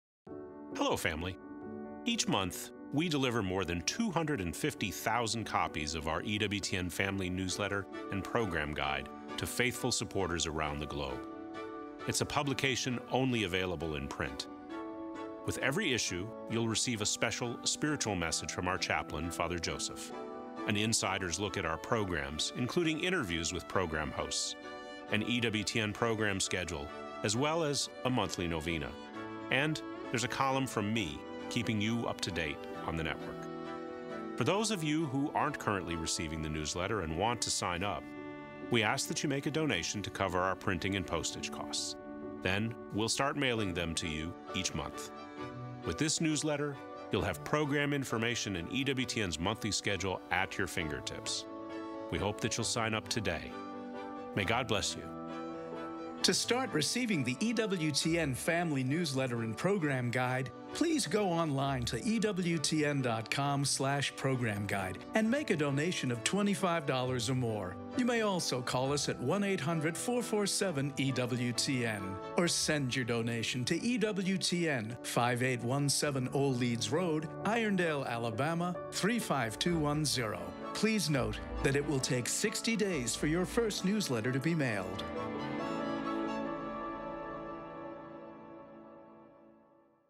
EWTN Newsletter and Program Guide Offer
Sign up for EWTN's monthly Family Newsletter and Program Guide for a donation of $25 or more.